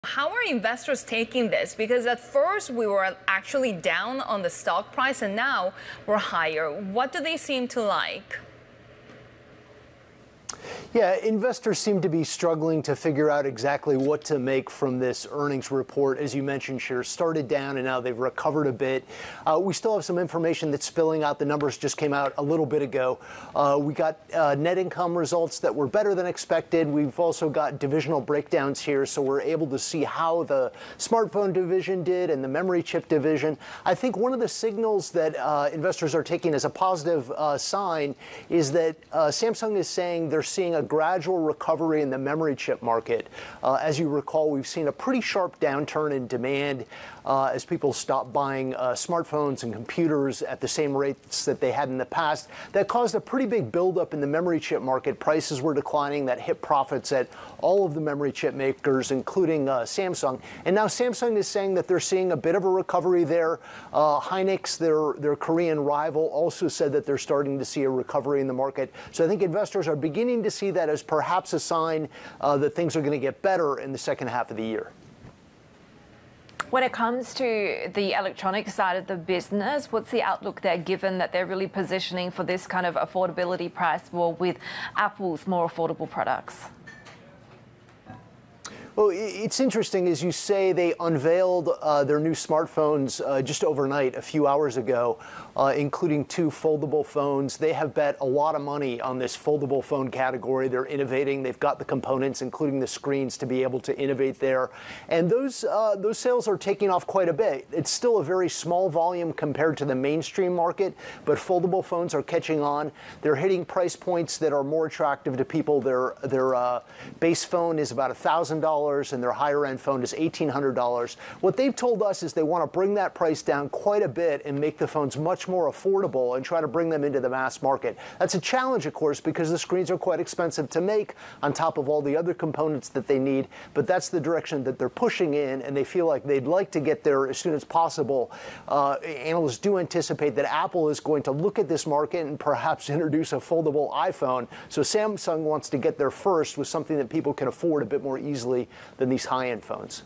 Samsung Electronics’ Profit Beats Estimates
Samsung Electronics Co. reported better-than-projected earnings. South Korea’s biggest firm, a bellwether for the tech industry because of its leading position in chips, electronics and smartphones, said it now expects a gradual second-half recovery in memory demand. Peter Elstrom reports on Bloomberg Television.